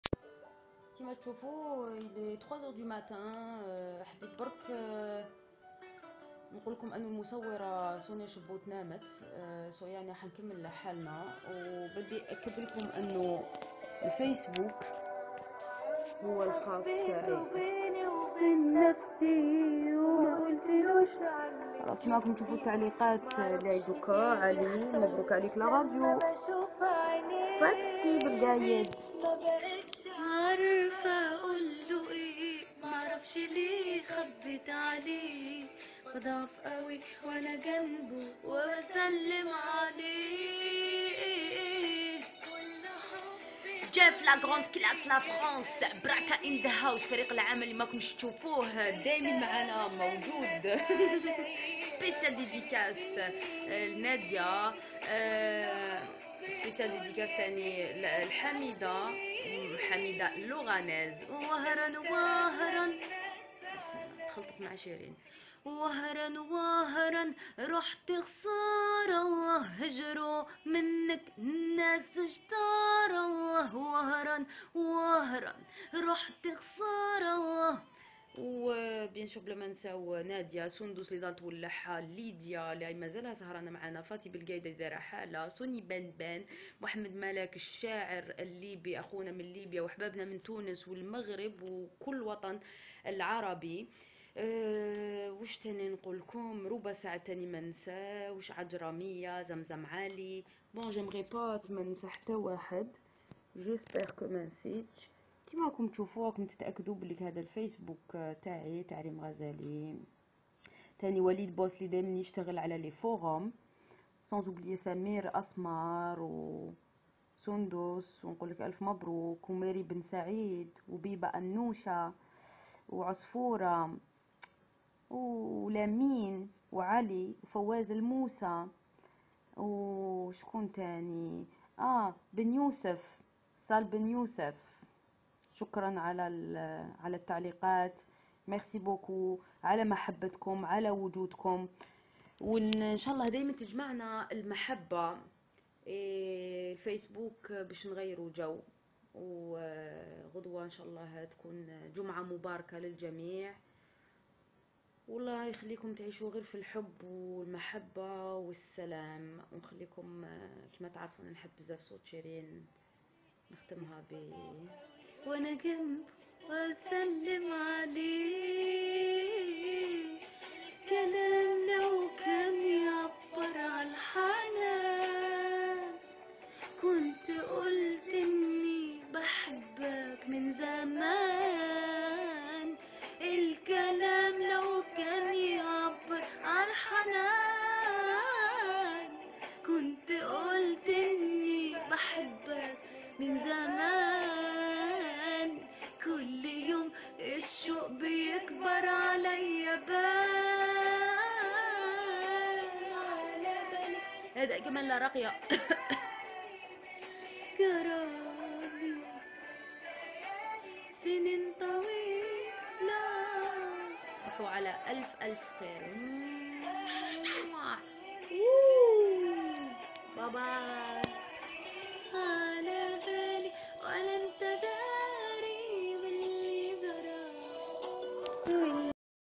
rym ghezali part 1.mp4
ريم غزالي تغني لفانزها وتبعثلهم اجمل تحيا عبر فايس بوك ريم غزالي تعشق جمهورها